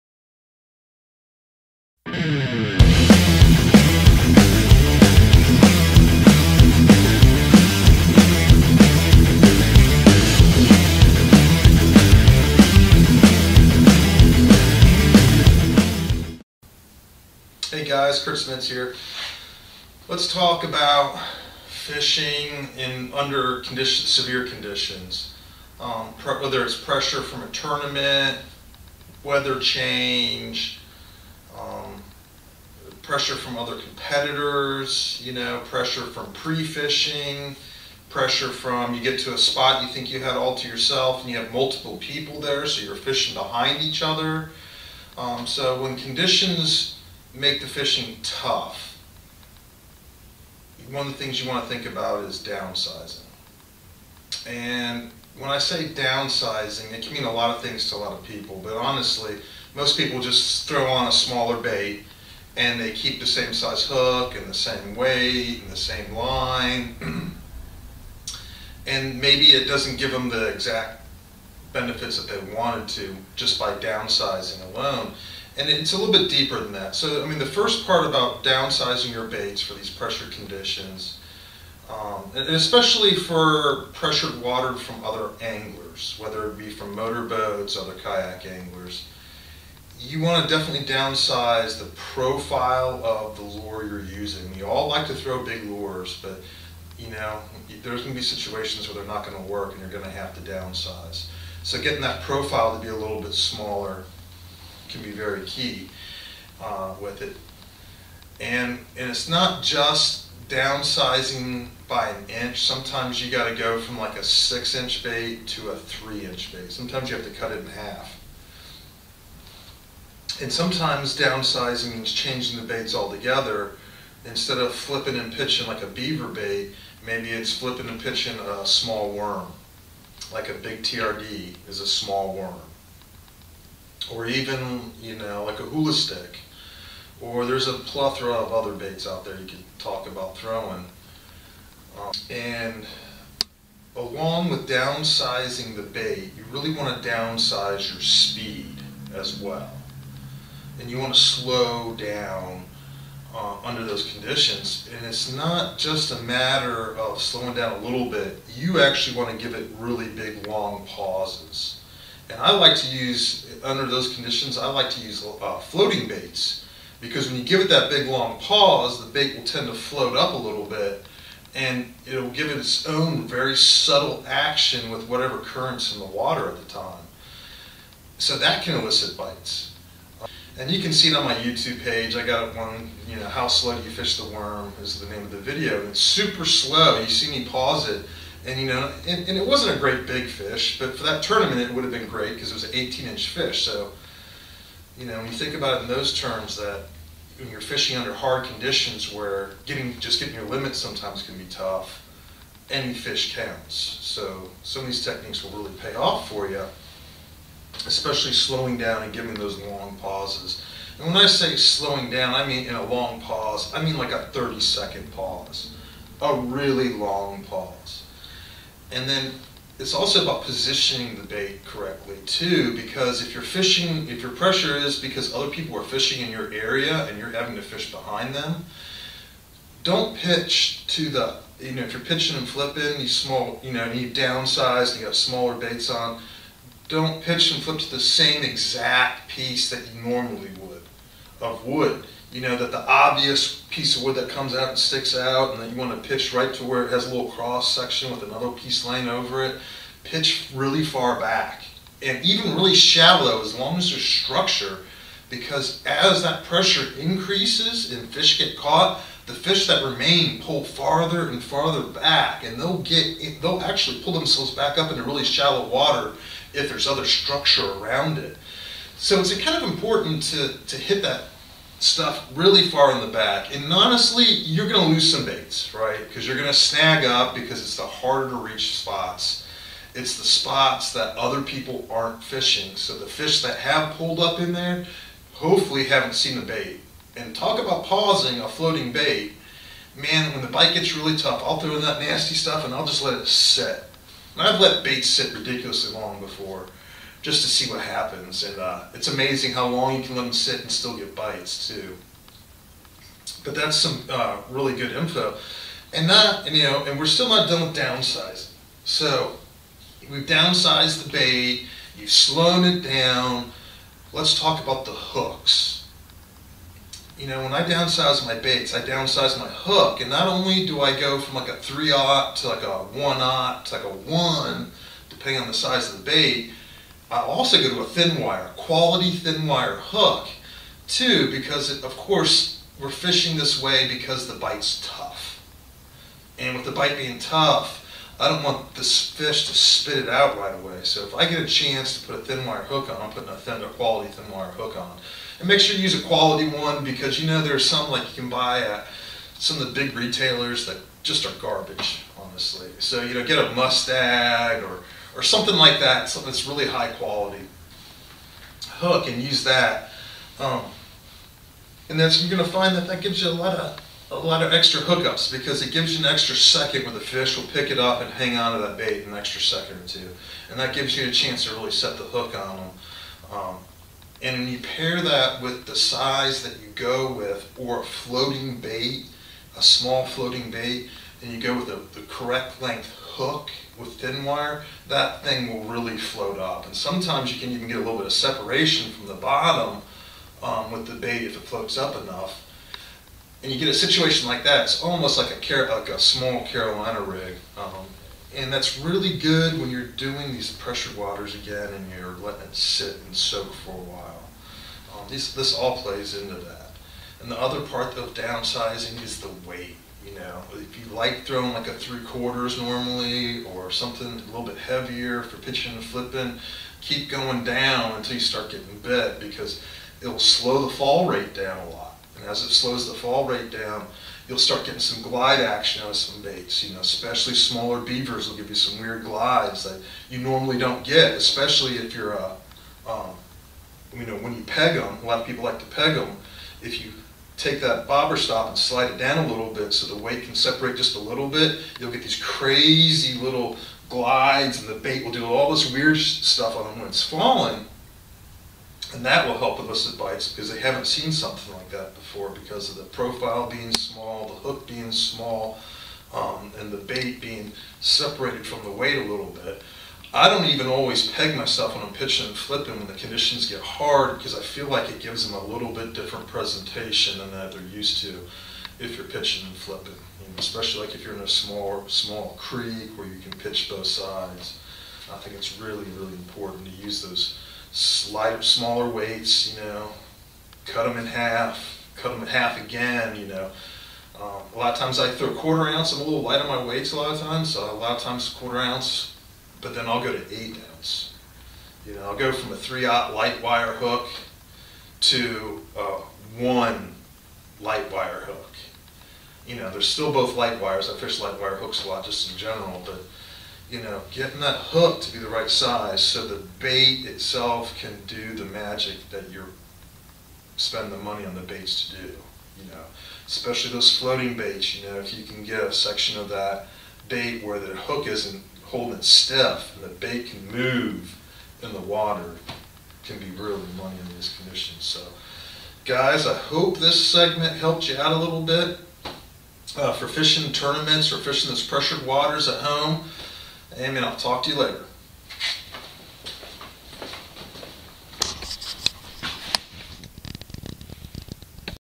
How to fish high pressured waters from a kayak
Have you ever gone to a tournament that had a lot of people signed up or on a weekend when everyone had the same idea to go fishing? In this video going over some ways to help you deal with pressured waters.